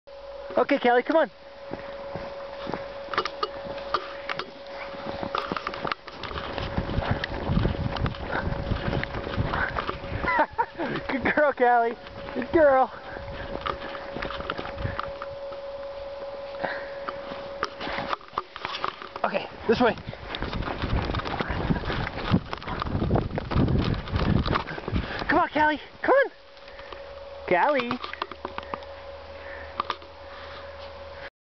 Callie in the snow at 16 weeks
Callie, the 16 week old Welsh Terrier, playing in the snow after a spring snowstorm.